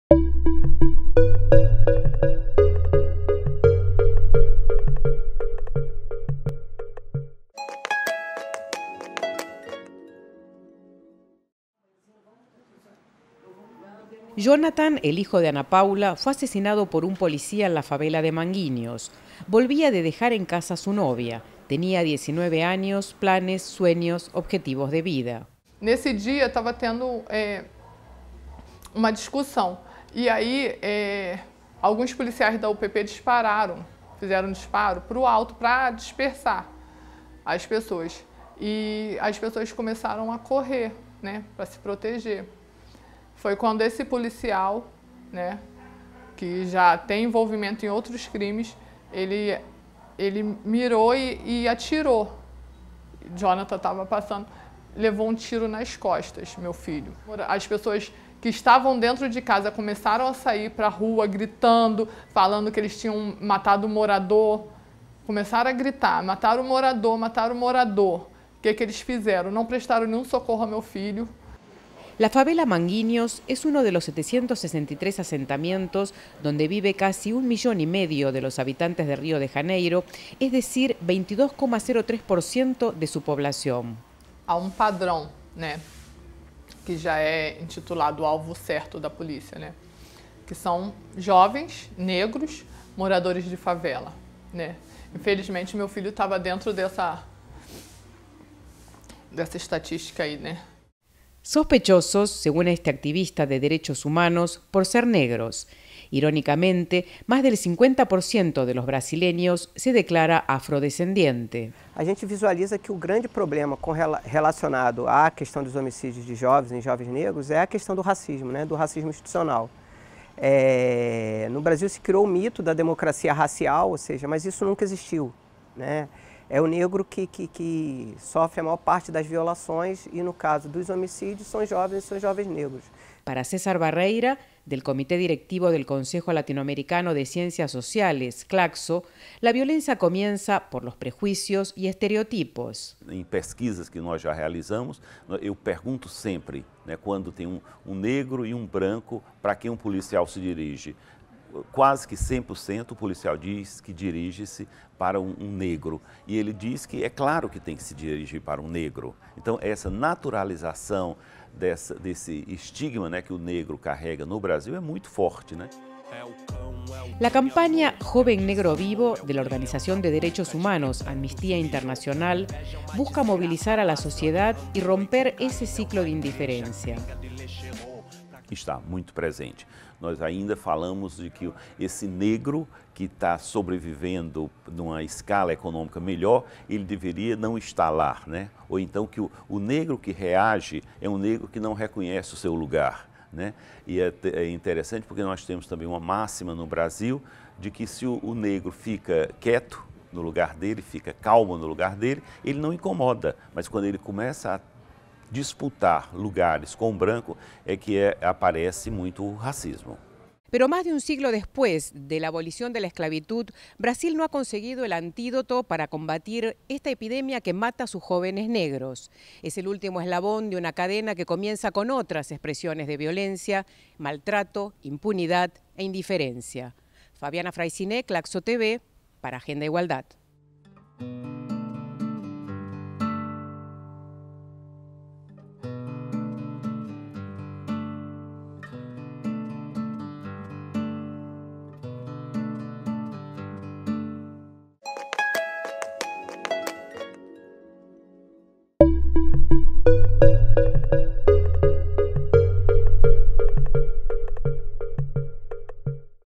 Agenda Igualdad - Río de Janeiro: jóvenes negros, violencia y discriminación
La violencia contra los jóvenes negros y de sectores populares en la ciudad de Río de Janeiro. Dilemas de la seguridad ciudadana en una de las principales ciudades latinoamericanas y palco de las Olimpíadas de 2016. Una producción de CLACSO TV para Agenda Igualdad (CLACSO / FLACSO Brasil). Agosto, 2016.